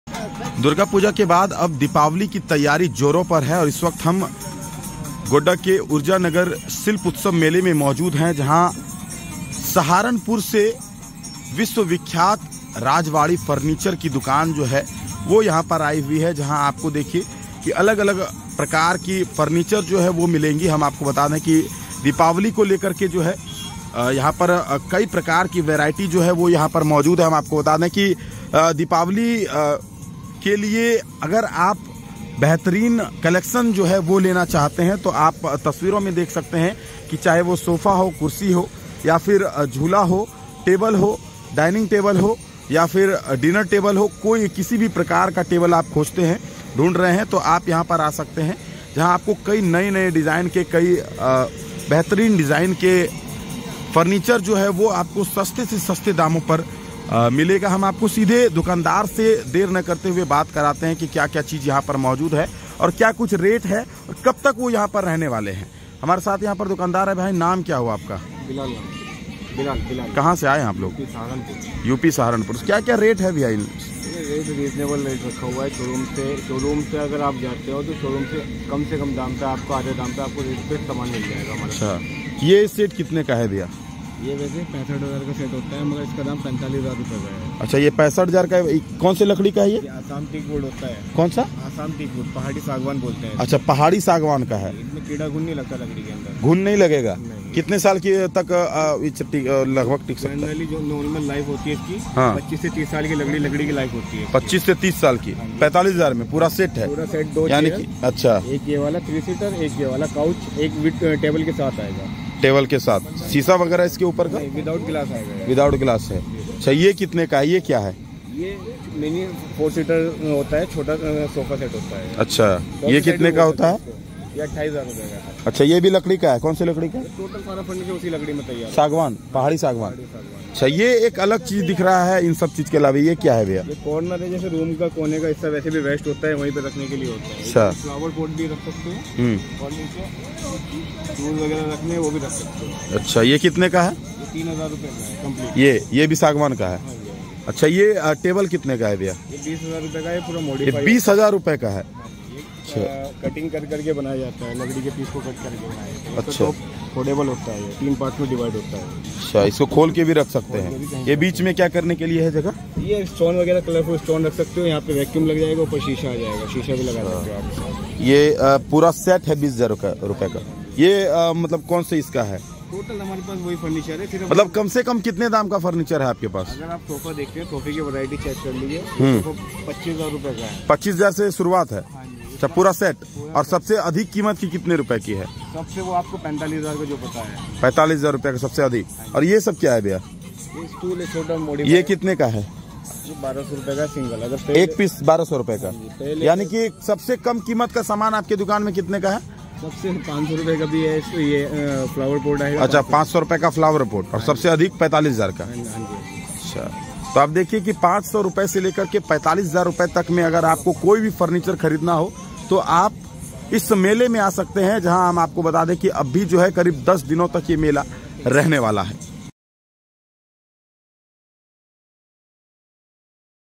इस जगह से कीजिए Diwali के लिए पहाड़ी सांगवान से बनी खास Furniture के लिए Shopping
फर्नीचर बेचने वाले बिलाल ने बताया कि वे लोग यूपी के सहारण पुर से आए हुए है और अब तक के मेले में अच्छी व्यापार की और अब और 10 दिनों तक इस बाजार में फर्नीचर की बिक्री करेंगे.ताकि लोग दिवाली की भी फर्नीचर खरीद पाए.

Furniture seller Bilal told that they have come from Saharanpur of UP and have done good business in the fair so far and will sell furniture in this market for 10 more days so that people can buy furniture for Diwali also. ..